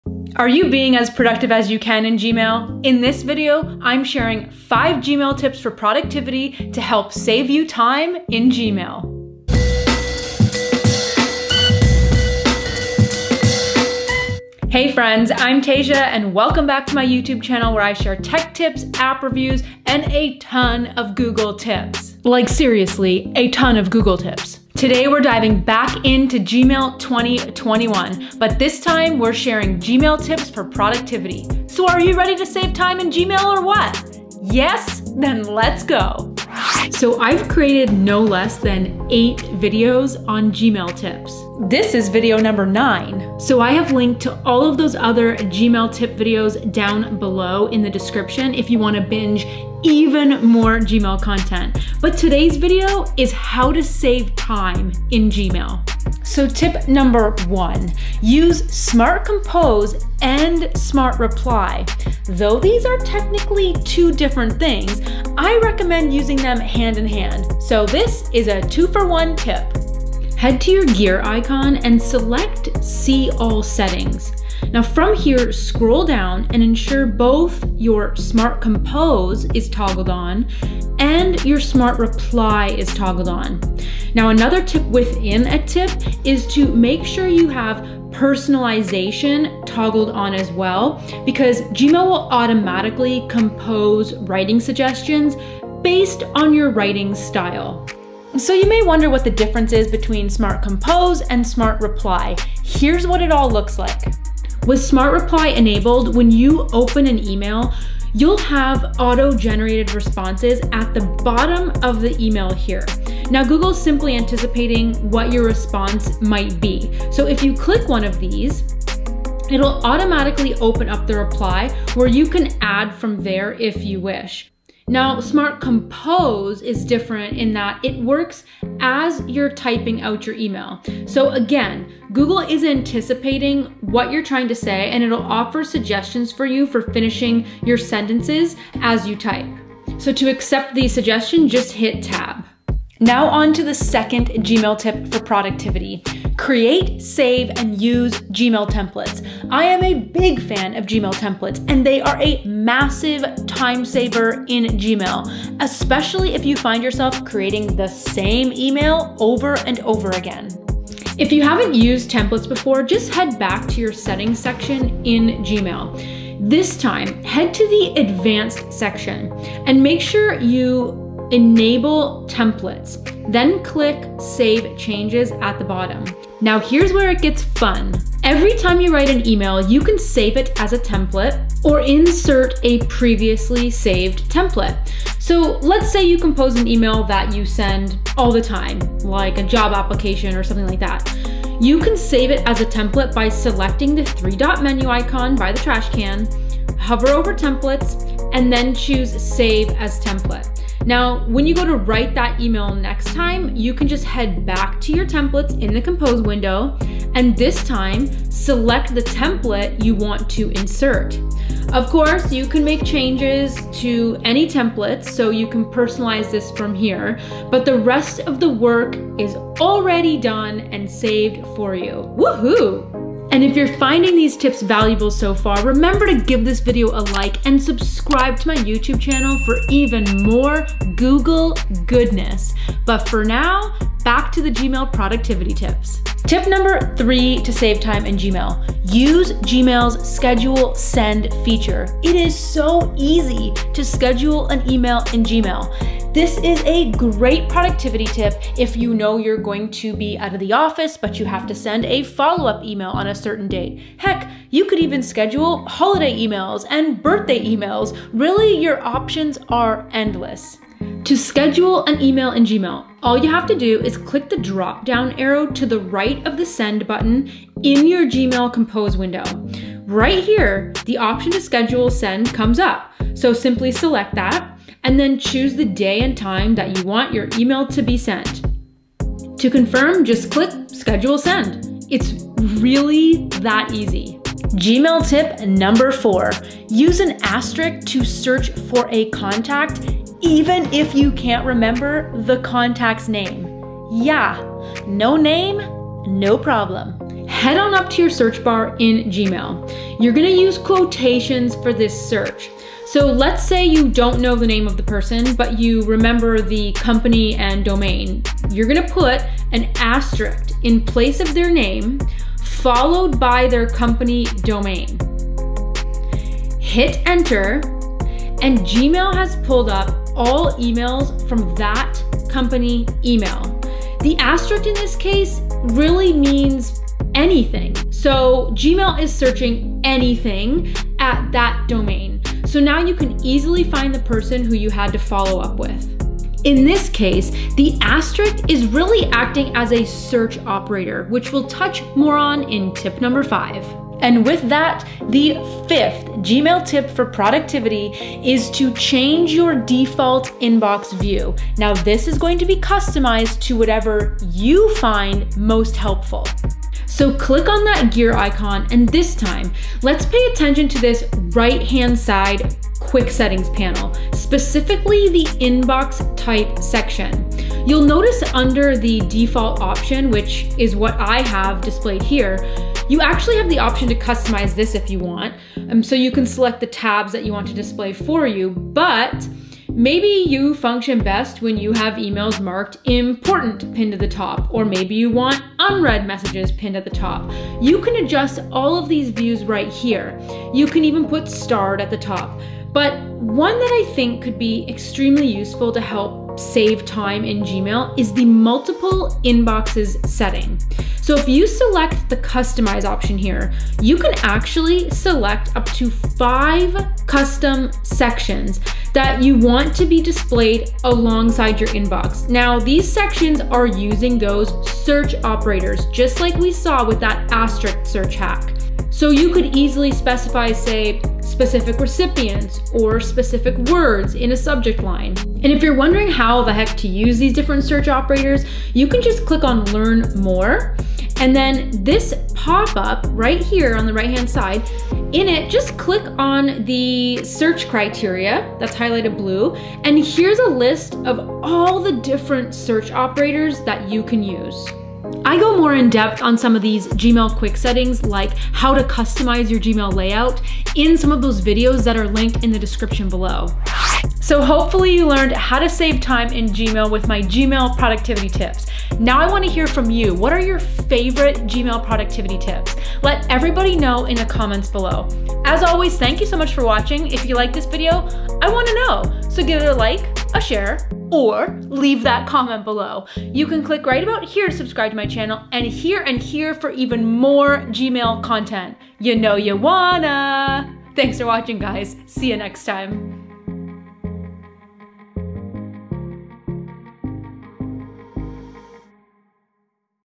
Gmail Tips: How to Save Time in Gmail | 5 Gmail Tips for Productivity (Gmail 2021)
Learn how to save time in Gmail with my 5 Gmail tips for productivity. Do you use Gmail? Are you as productive as you could be with it?

In this video, I’m sharing some of the best things about Gmail 2021, including my top 5 Gmail tips for productivity. You’ll learn how to save time in Gmail by utilizing features like smart compose, schedule send, templates, search operators, and more! Get ready to spend less time in Gmail!

⏰ TIMECODES:
00:00 - Intro
00:37 - Smart Compose & Smart Reply
02:23 - Gmail Templates
03:56 - Schedule an Email in Gmail
04:45 - Use * For Smart Search
05:44 - Change Default Inbox View

✅ RECOMMENDATIONS:
🎞 I use Camtasia for all my screencasting needs